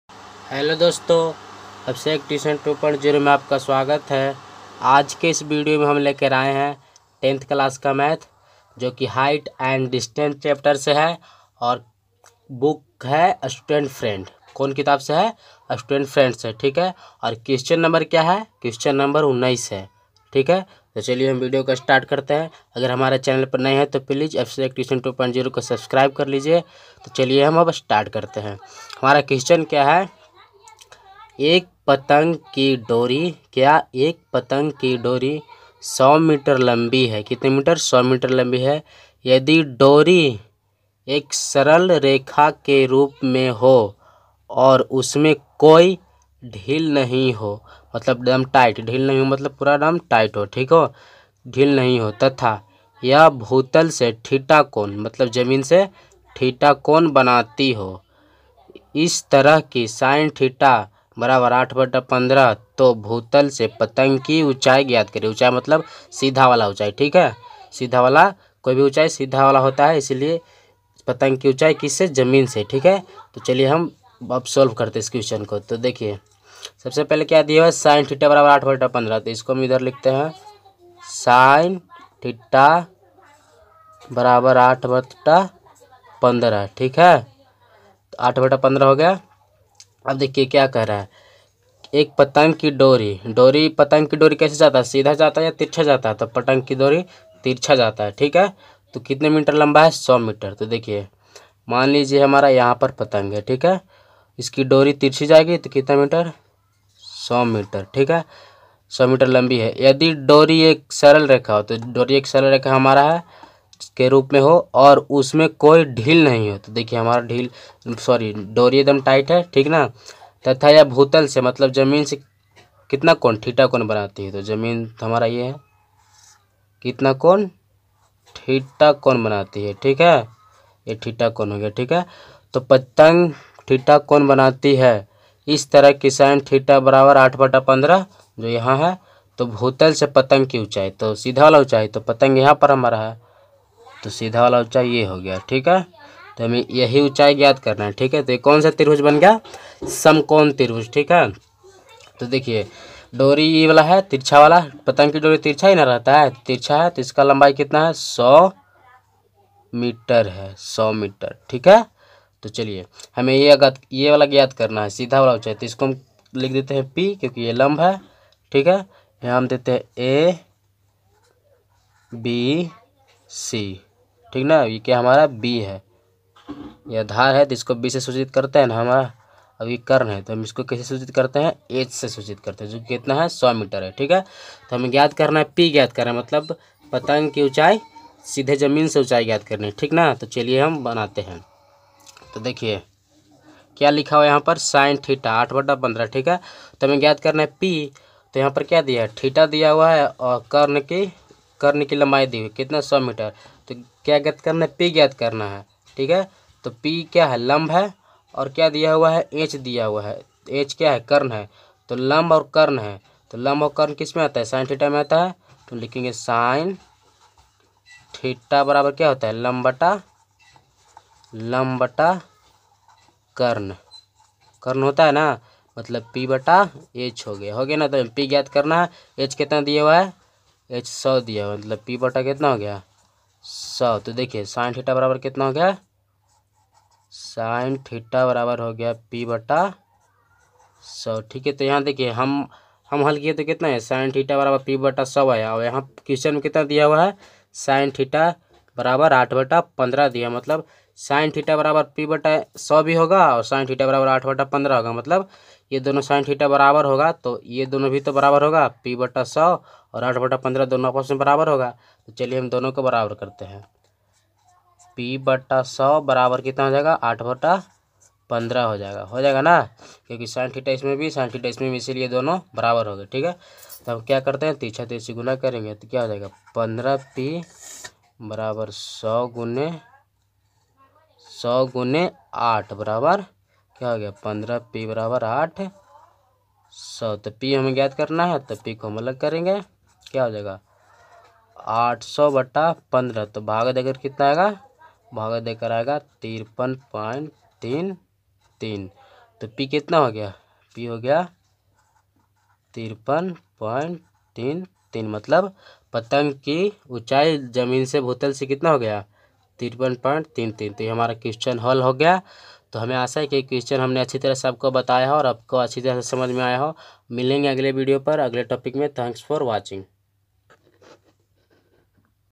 High and distance chapter question number 19 student friend book class 10
Hallo dosto aaj ke इस video me hum high and distance chapter ke question number 19 ko solve karenge student friend book se
Class10 math
High and distance
Chapter
Abhishek tution2.0